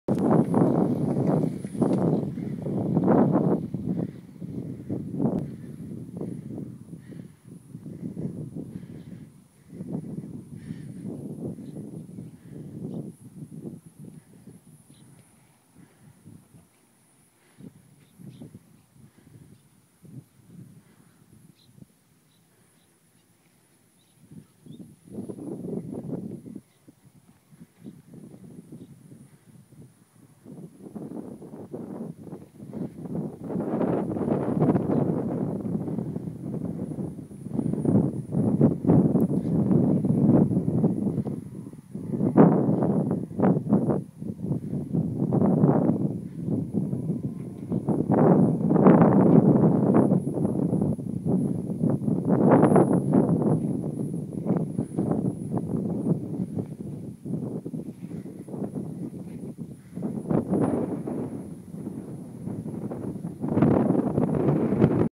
Seashell fossil found at 1000 ft altitude (3of3) 720p HD
While hiking in Laguna Coast Wilderness park in Southern California, I observed this seashell fossil at about 1000 ft in altitude. Nearby, in Laguna Niguel, at this approximate altitude there is a prehistoric beach that has been dated to approximately ten million years ago; so this fossil is probably about ten million years old. I will also be posting higher resolution photos on Flicker and I will add the link on here soon.